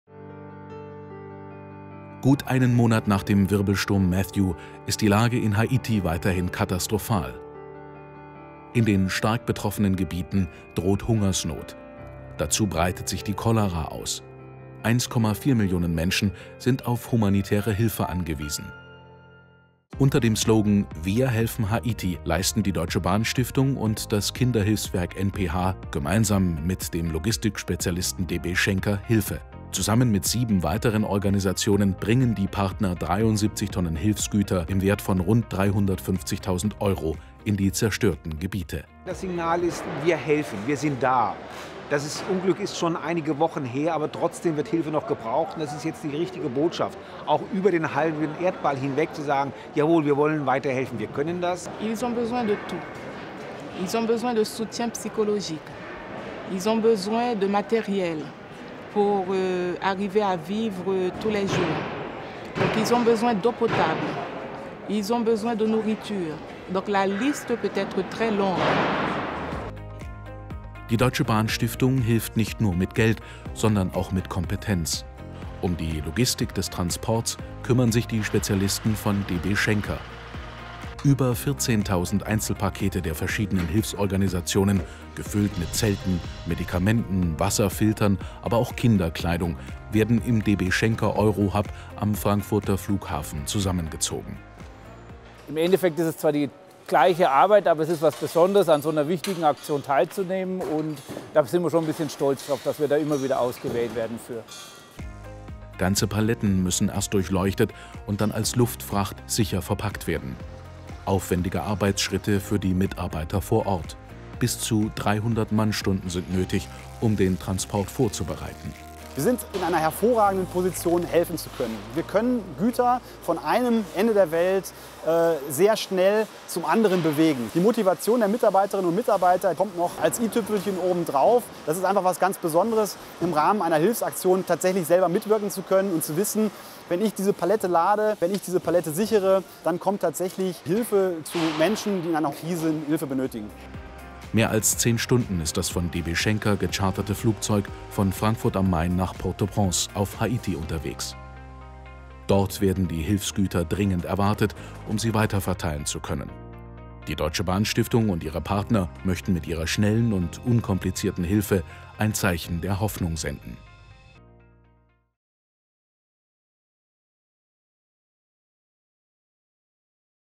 Deutsche Bahn Stiftung: Wir helfen Haiti
Gut einen Monat nach den verheerenden Auswirkungen des Wirbelsturms „Matthew“ bleibt die Lage in Haiti katastrophal. Es droht eine Hungersnot und Cholera breitet sich in den betroffenen Gebieten aus. Vor allem Medikamente werden dringend benötigt. Wir helfen mit und bringen durch die Deutsche Bahn Stiftung sowie der logistischen Expertise von DB Schenker 73 Tonnen Hilfsgüter von Frankfurt am Main nach Port-au-Prince.